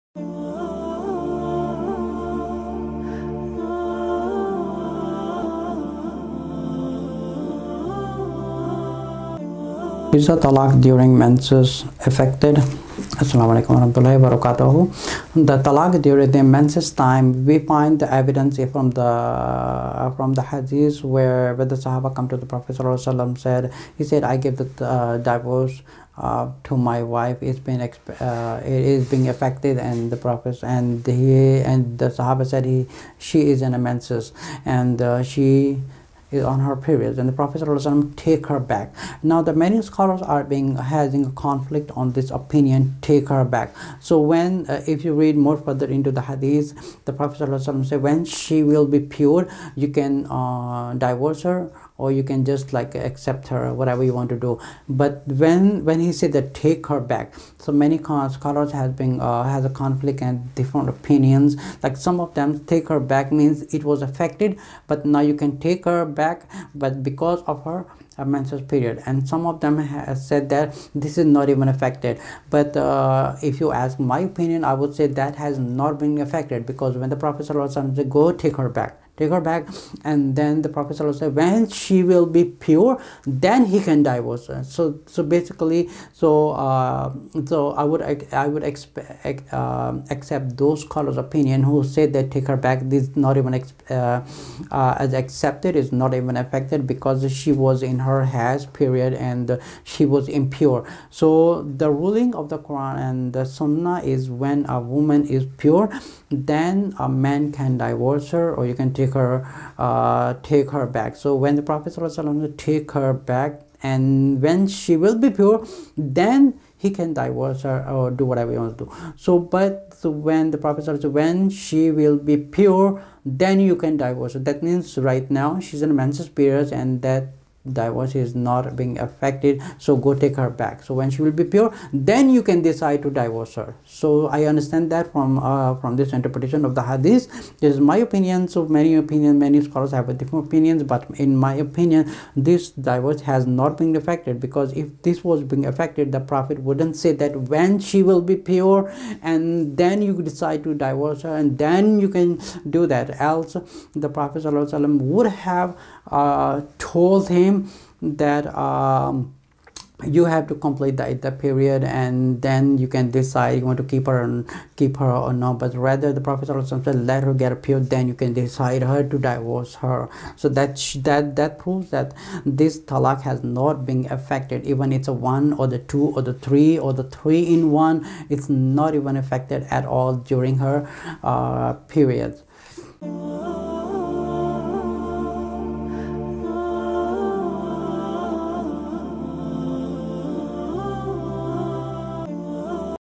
Husband Give Talaq During Her Monthly Periods ~Mufti Ammaar Saeed
Mufti Ammaar Saeed
IRFNY